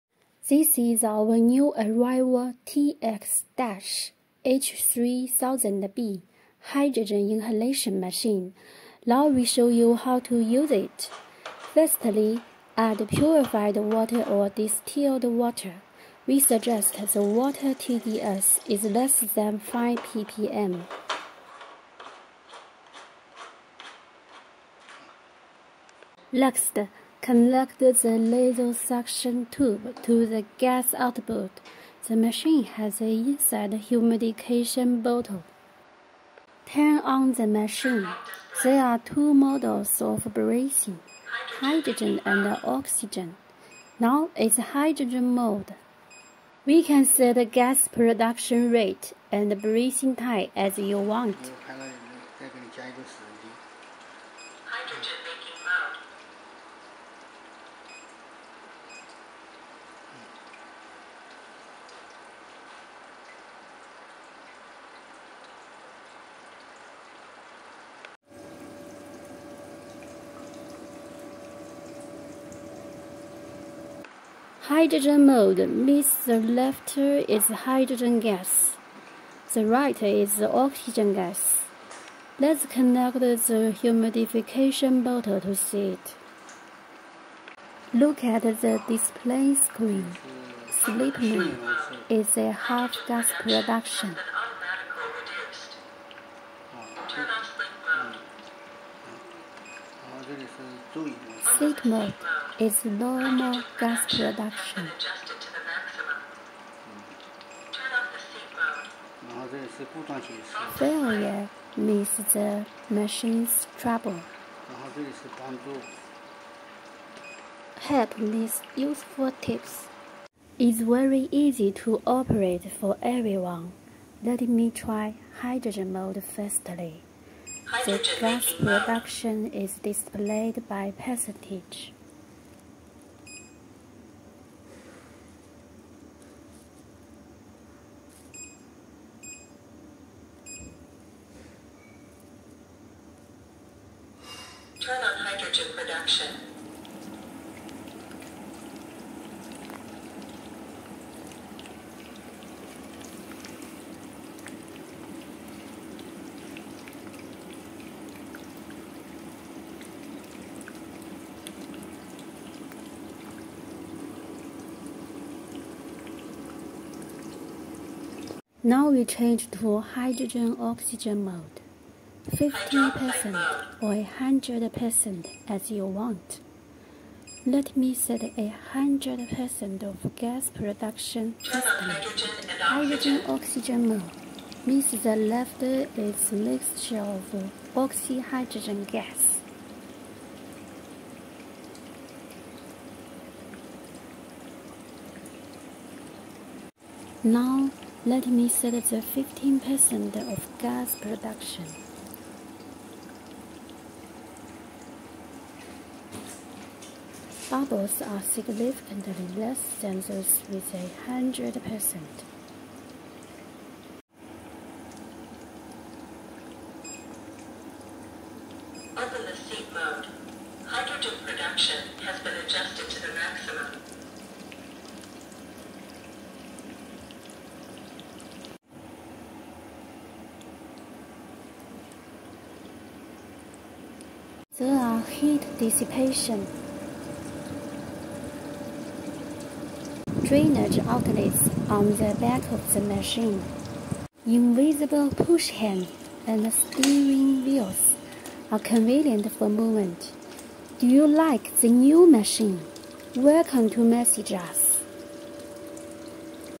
How to use hydrogen inhalation machine TX-H3000B -new arrival
Today we introduce you the new design TX-H3000B hydrogen inhalation machine and how to use it.